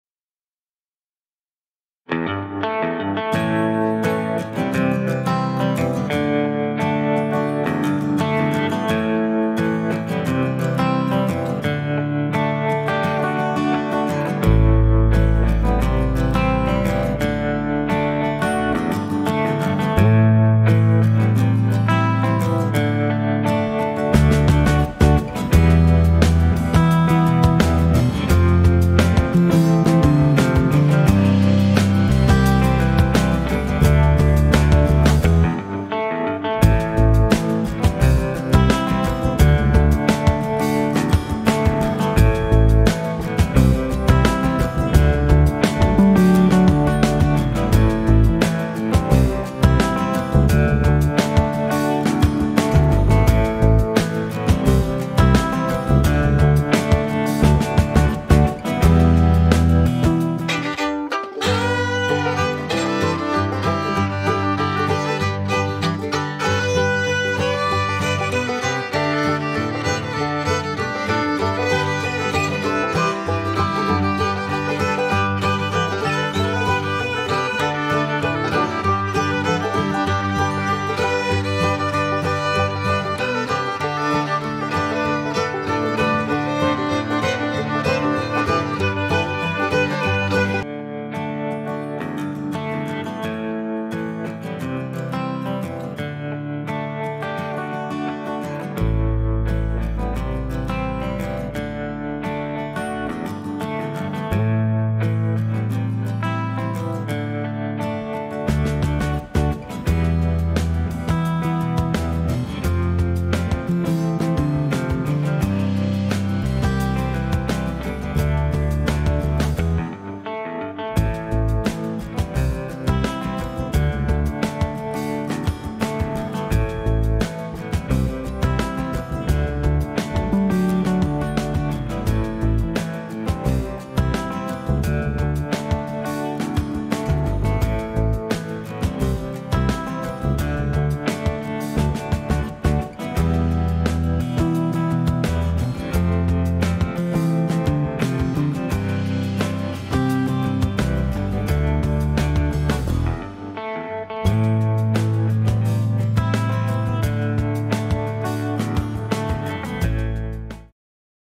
Elegantiškas medžioklės bokštas su stogu! Medžiotojos dienoraštis #4
Medžioklės bokštų esmė ta, kad jų niekada nebūna per daug. Naujos teritorijos, naujos galimybės ir naujas medžioklės bokštas!